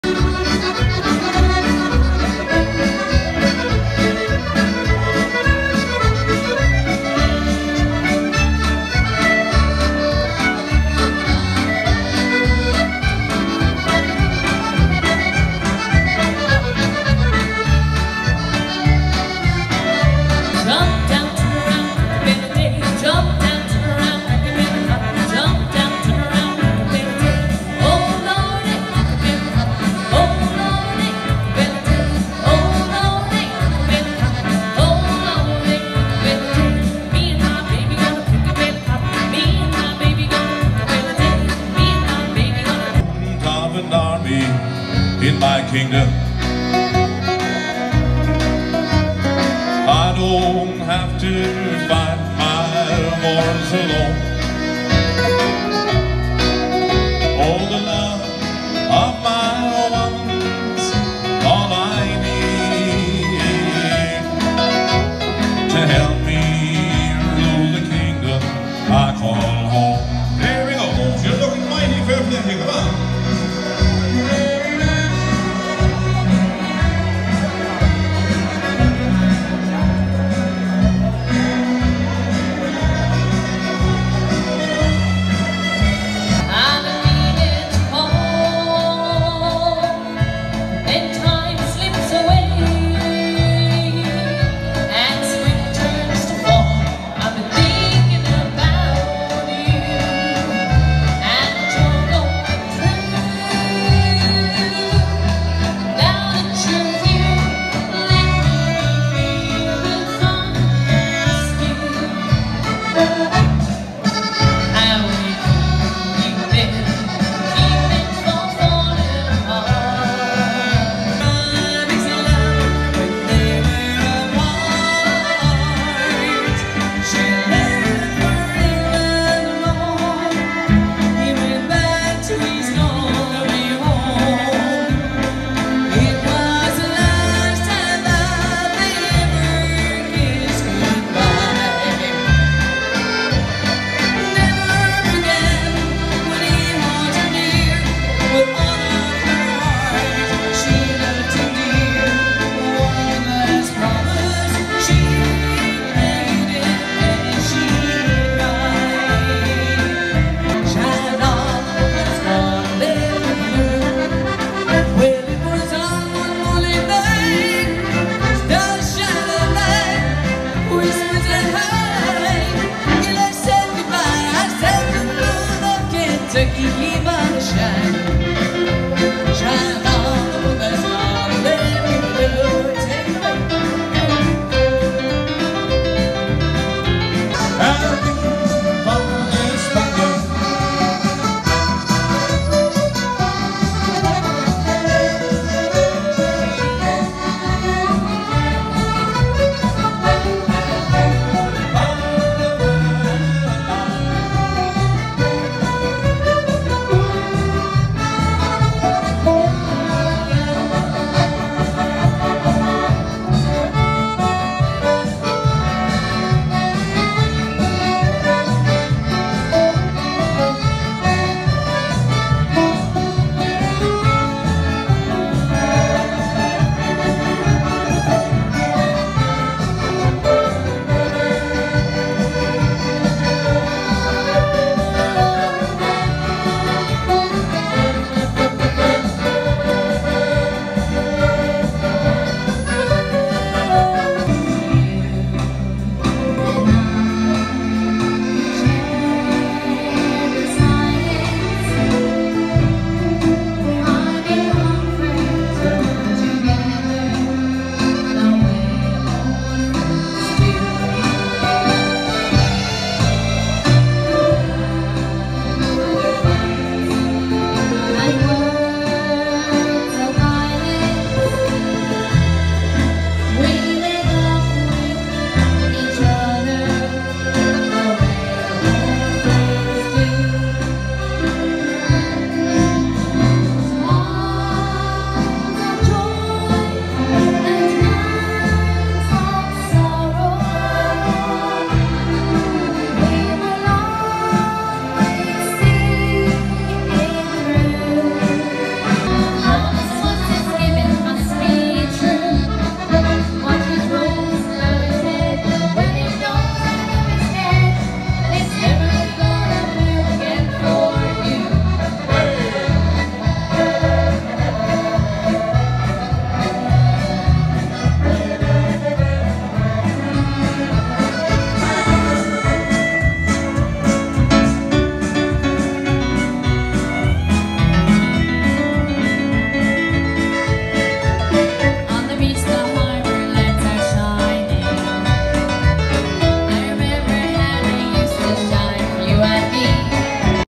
The Finbarr Dennehy Band
Finbarr and the girls had their own show in the Firgrove Hotel in Mitchelstown Co Cork
There were so many requests to do one after their hugely popular Facebook Live shows during lockdown
They were joined in the night by Martin Kearney who is a fantastic 5 row player
Special guest was Louise Morrissey a lady well known on National and international stages
This video is just short snippets of what was an amazing night of music, singing and dancing as over 250 people packed into the hall